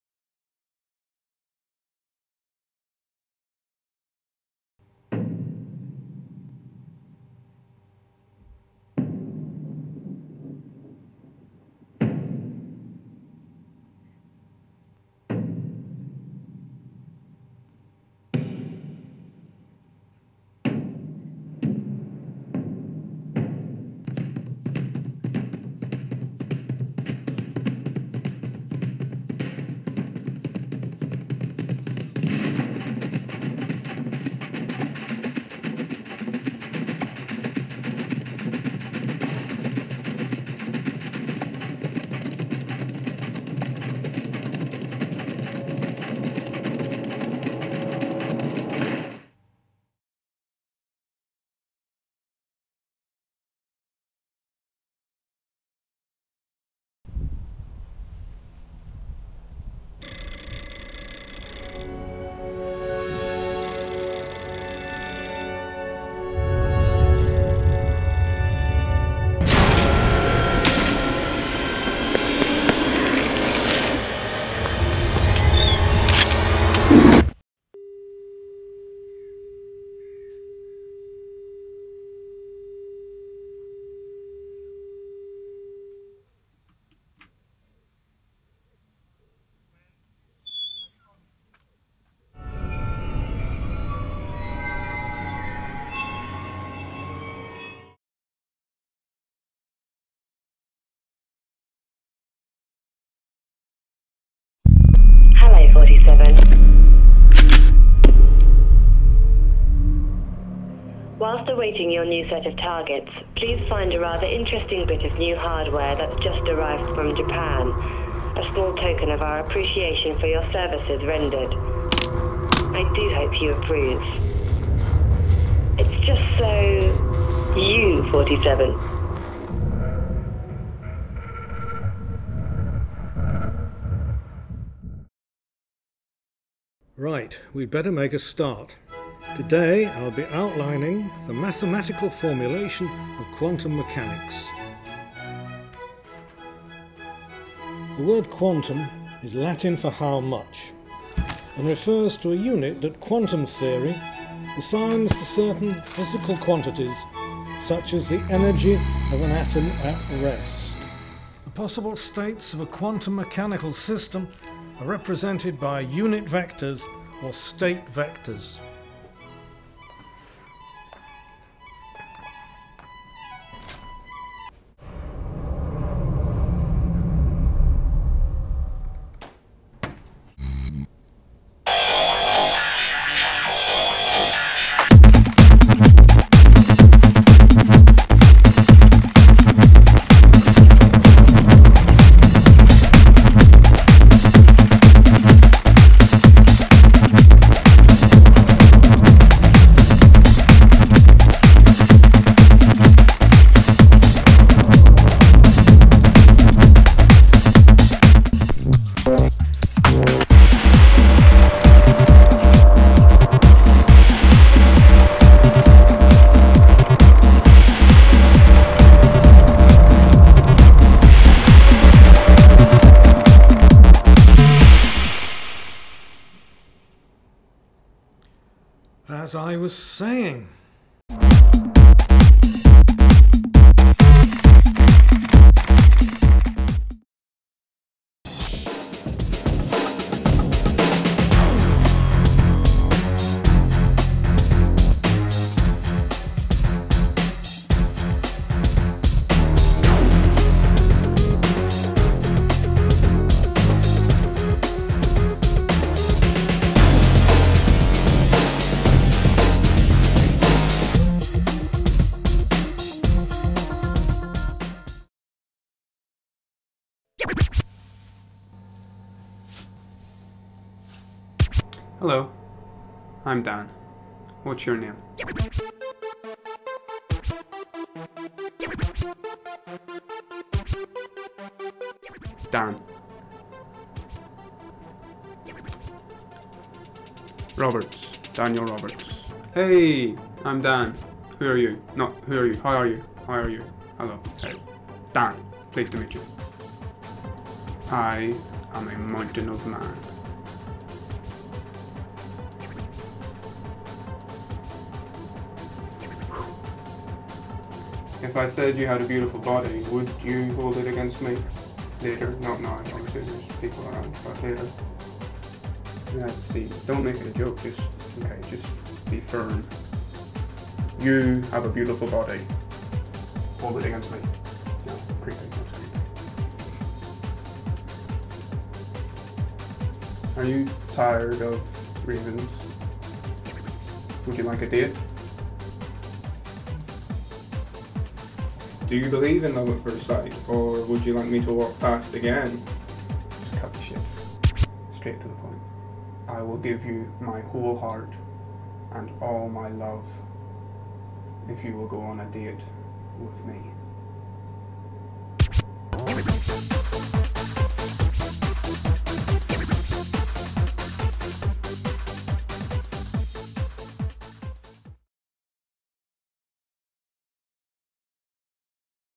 Lee Francis- Director's Showreel
A selection of my various honours projects- promos, edits etc- shot on DV with no budgets. Hopefully it provides a flavour of my style and interests that I'm looking to develop further in my career.